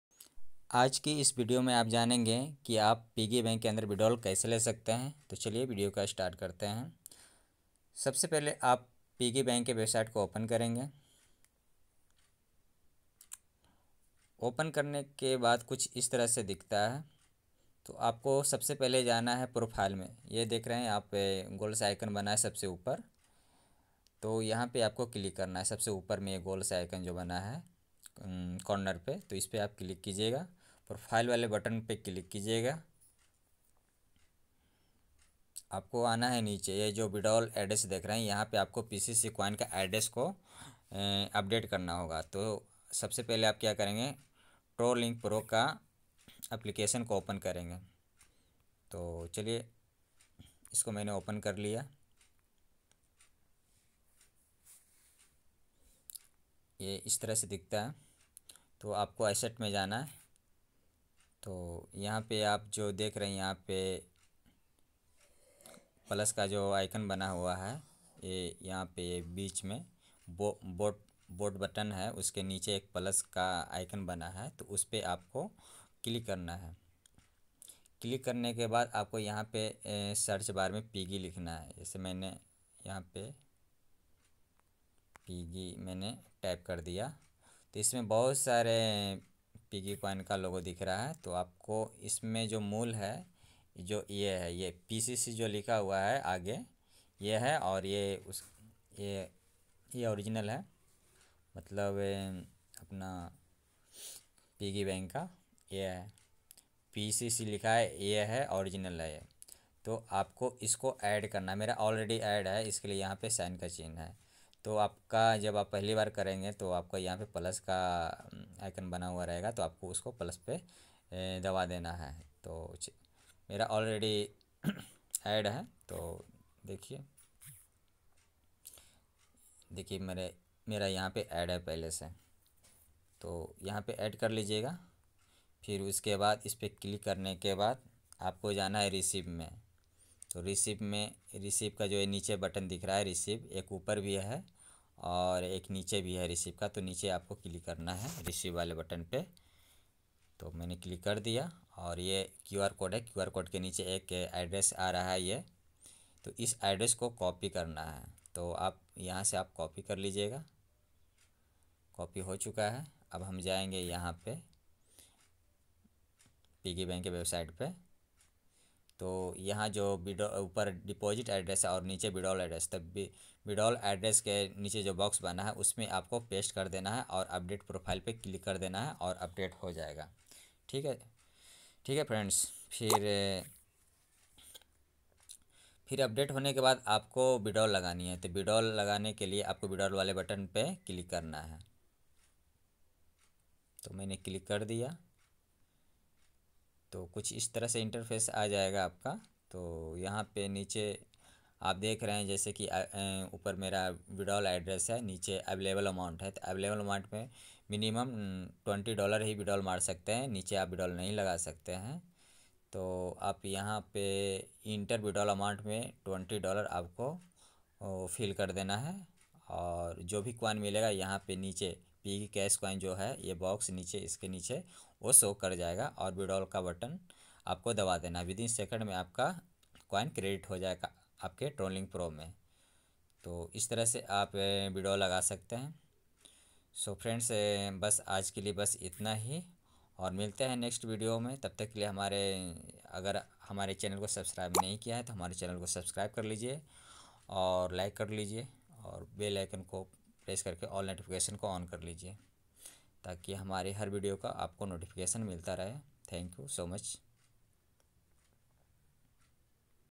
Piggybank me withdrawal kaise karen.|How to withdrawal in piggybank.
Aaj ki iss video me aap janenge ki piggy bank me withdrawal kaise karte hain.

Piggybank

Piggybank earning app

Piggybank me withdrawal kaise lagaye